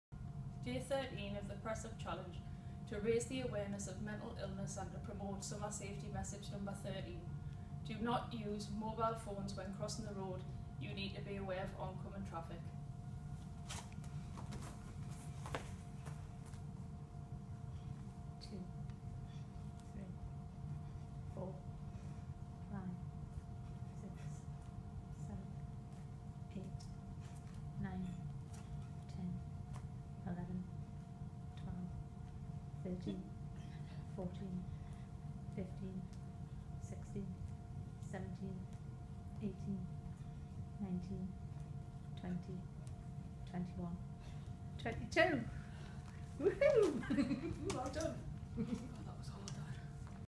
TWFRS Day 13 22 Push-up Challenge
Day 13  of the 22 push up challenge!

Tyne and Wear Fire and  Rescue Service would like to support the 22 push-up challenge to raise awareness of PTSD amongst service men and women.

In support of this initiative and alongside the Mind Blue Light Programme which also supports and challenges the mental health stigma, Tyne and Wear Fire and Rescue Service would like to deliver a different Summer Safety messages each day to ensure that our communities have a safe and enjoyable summer.